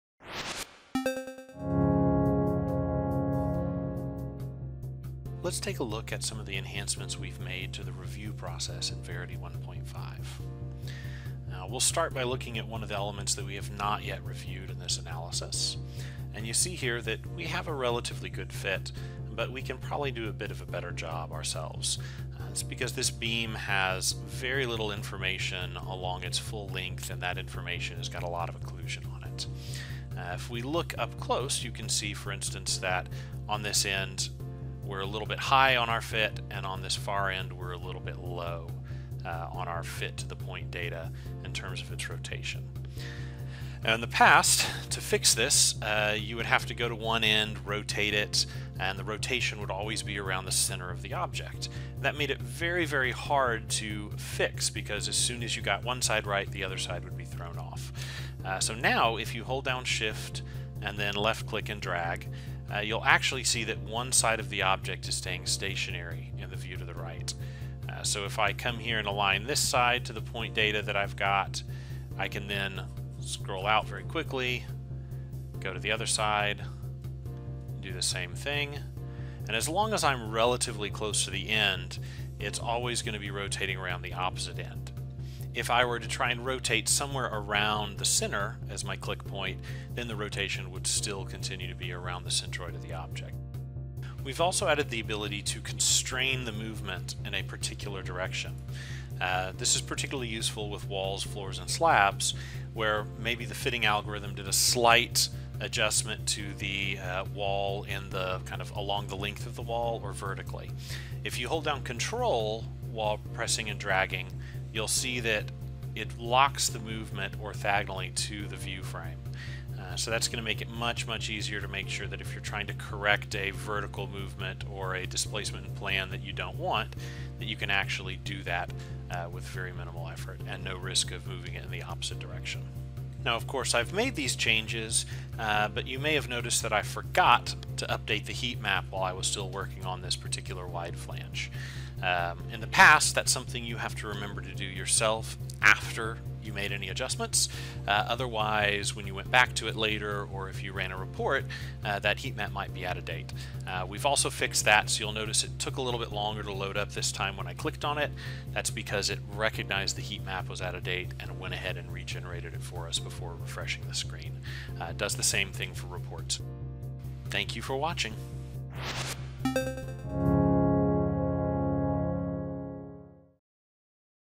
Verity 1.5 Feature Preview - Review Enhancements
Verity compares point clouds against design and fabrication models, allowing 100% verification of work in the time it currently takes to spot check 5%. Verity software finds construction mistakes before they become expensive problems.

Made adjusting the fit of your items to the point cloud more efficient with a more refined rotation tool and axis constrained movement, and we’ve made heatmap regeneration automatic upon returning to the item or generating reports!